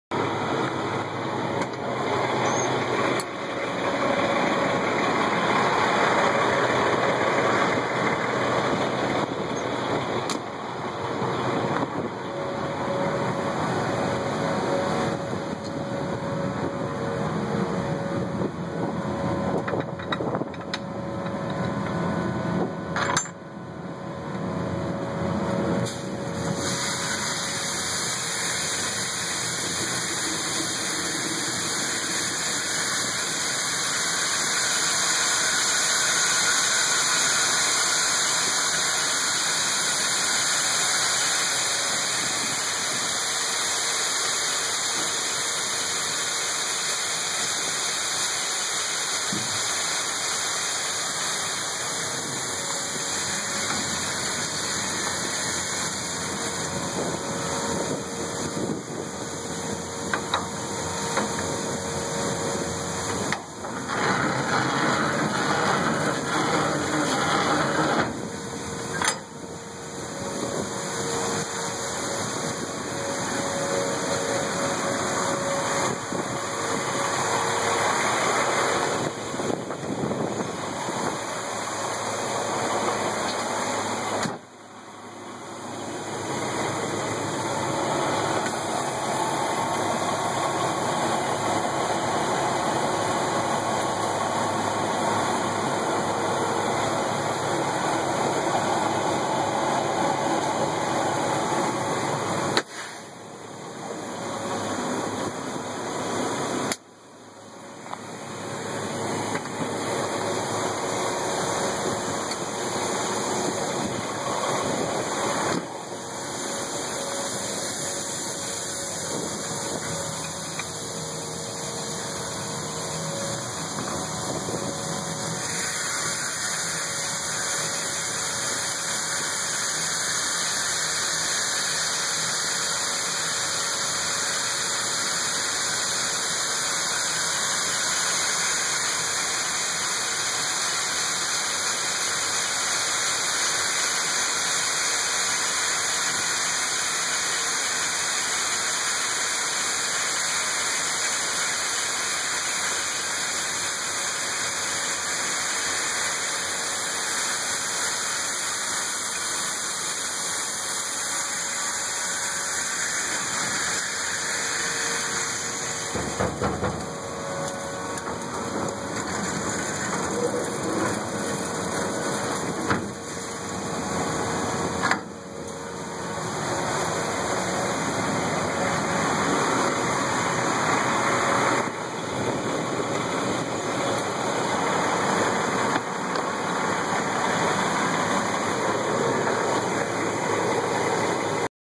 1000 Bu unloaded at elevator
Unloading 1000 bu grain at elevator with 378 Peterbilt and Wilson Grain Trailer.